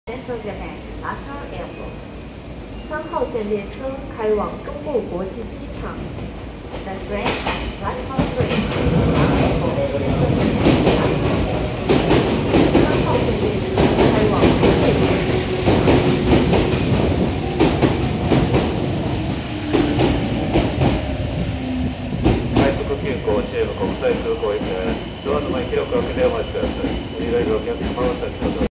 ●20220524 3500×3100系 到着 ＠金山
撮影路線：名鉄 名古屋本線
撮影駅：金山
行先：快速急行 中部国際空港
車両：3500×3100系
両数：4+2両＝6両
MH：ーー
備考欄：